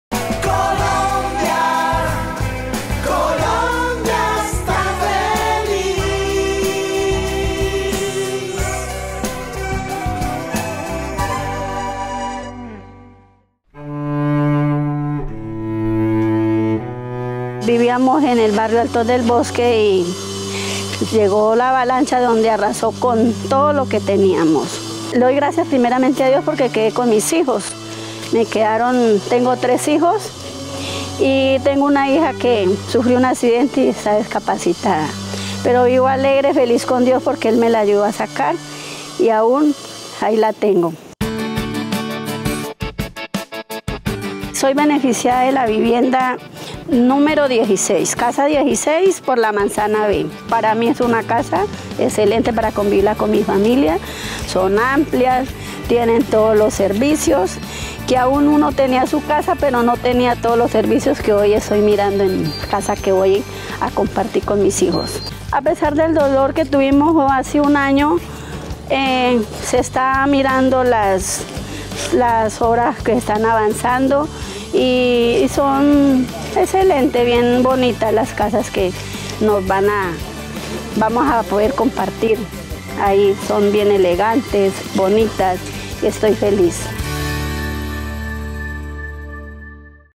Viviendas Dignas con servicios públicos para damnificados de Mocoa
Carme Ligia Ortega beneficiaria de la Ciudadela Los Sauces I en Mocoa, goza de su nuevo hogar. Una vivienda digna con todos los servicios públicos.